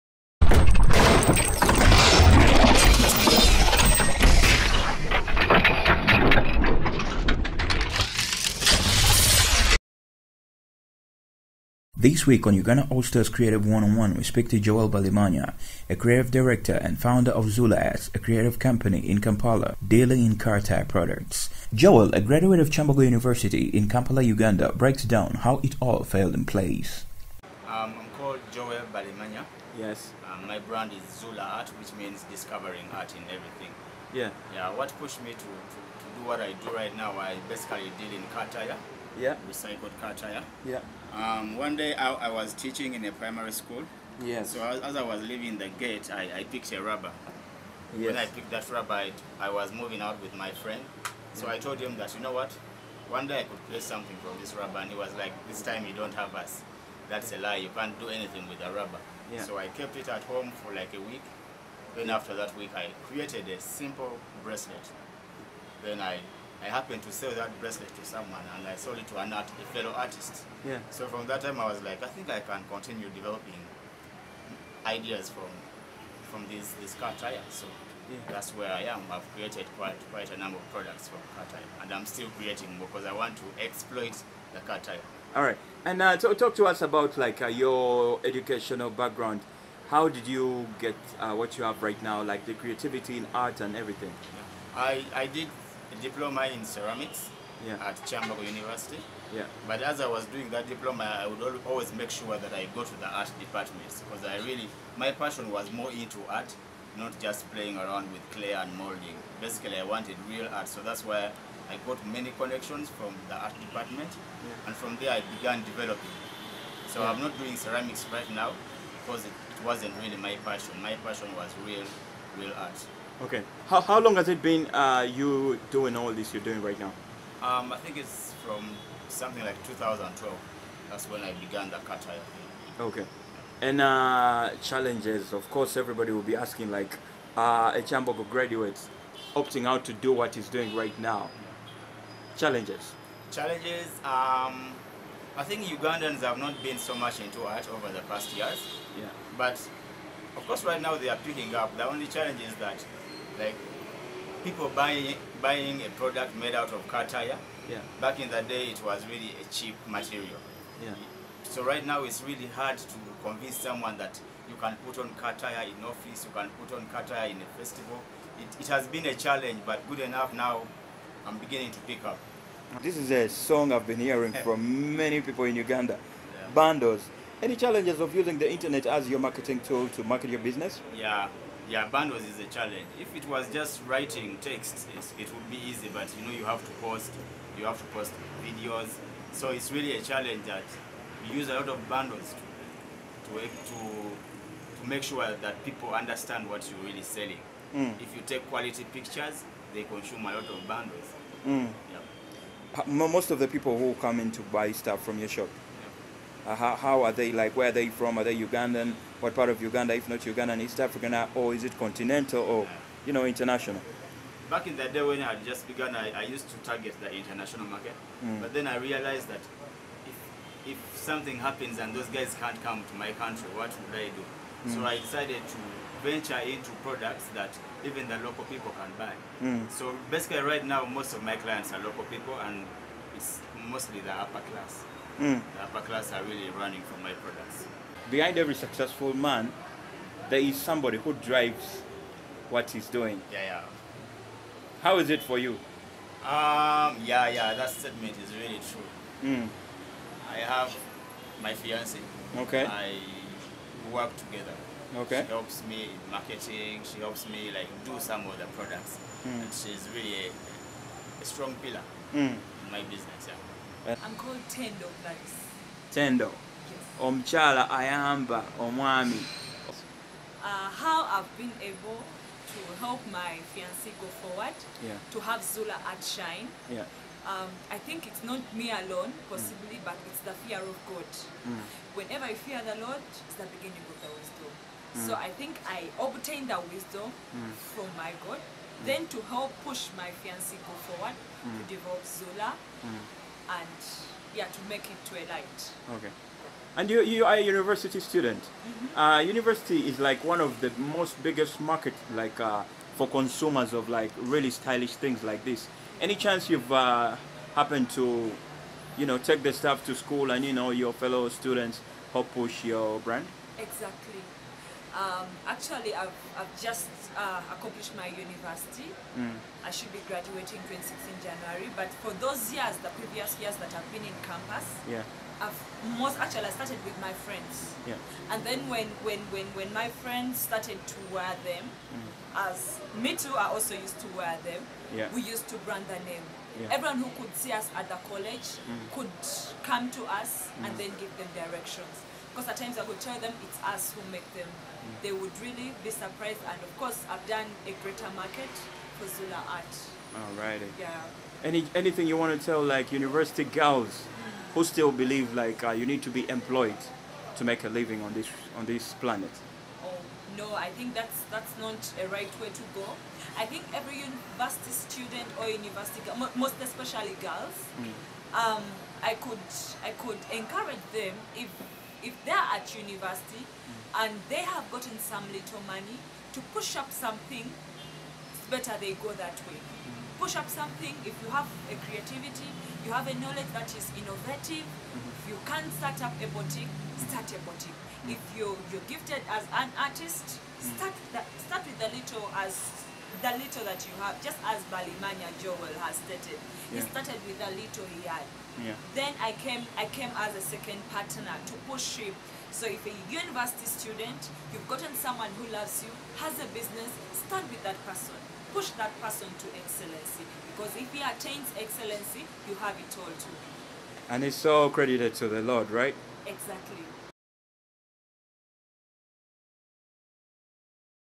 JOEL BALIMANYA ARTIST UNIVERSITY GRADUATE EARNING A LIVING FROM TYRE PRODUCTS
Eco Art as a Career.